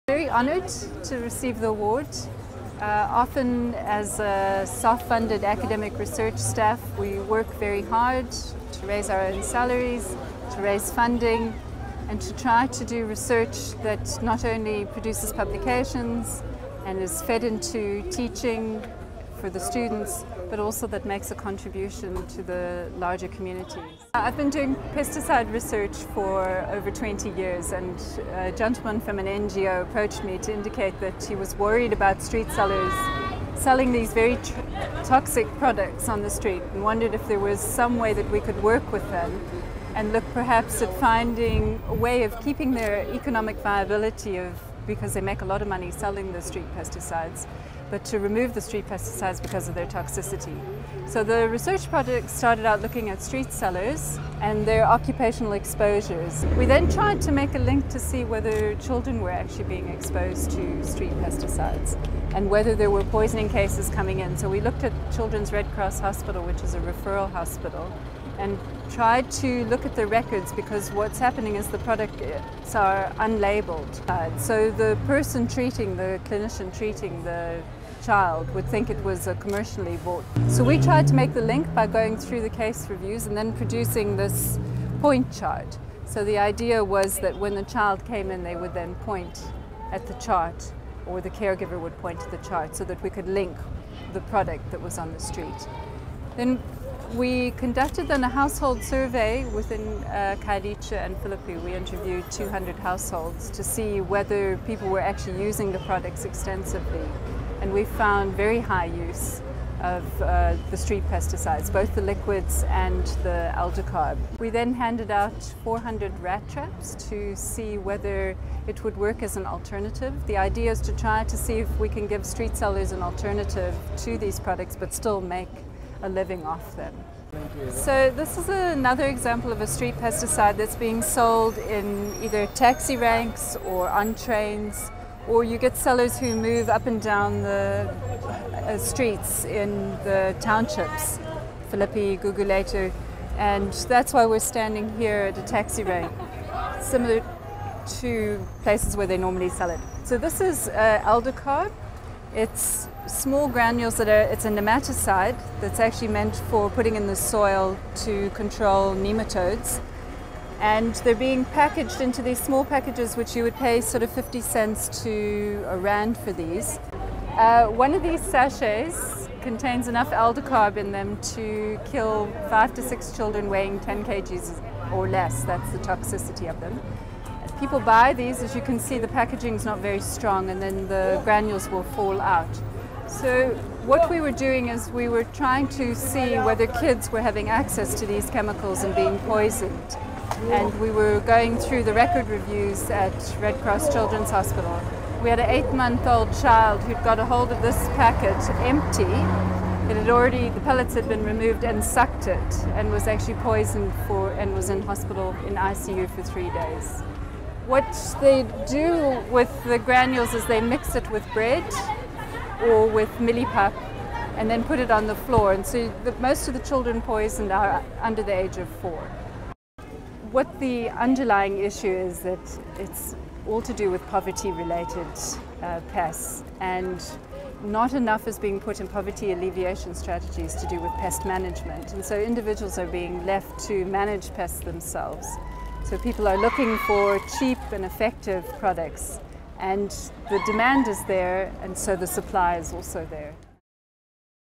Consequences of llegal toxic pesticides among the poor in peri-urban communities.
Dr Hanna-Andrea Rother (programme leader of Health Risk Management in the Centre for Occupational and Environmental Health Research at the University of Cape Town) was the recipient of the UCT 2010 Distinguished Social Responsiveness Award. She speaks here about her work on the consequences of the use of illegal and highly toxic street pesticides on children's health and health policy.